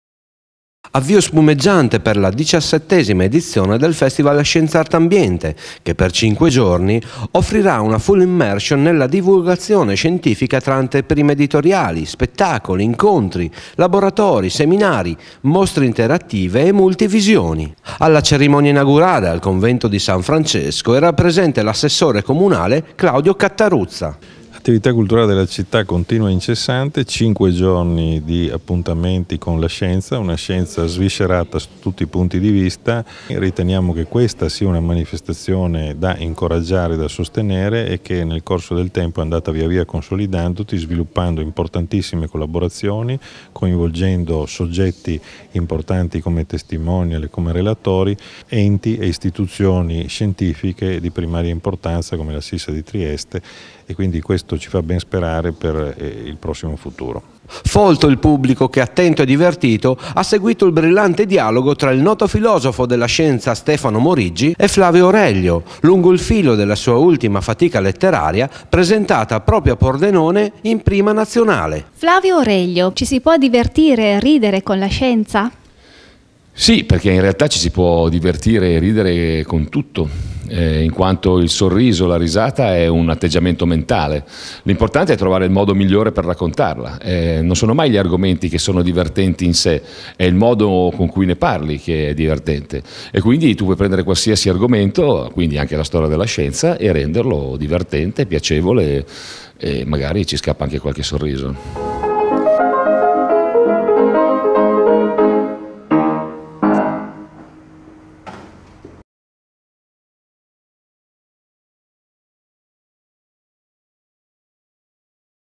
Pordenone: inaugurato Scienzartambiente - per un mondo di pace
Primo festival nella regione Friuli Venezia Giulia dedicato alla divulgazione e comunicazione scientifica.
Promosso dal Comune di Pordenone - Assessorato alla Cultura e dal Science Centre Immaginario Scientifico con la collaborazione di Istituti, Aziende ed Enti del territorio regionale.